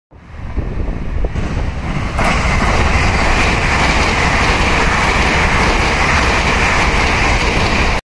2019/12/02 136次普悠瑪自強號大湖通過
編組: 40TED2006-40TEMA2011-45TEP2006-40TEMB2012-40TEMB2010-45TEP2005-40TEMA2009-40TED2005 (配置樹林)
路線起迄: 潮州-南港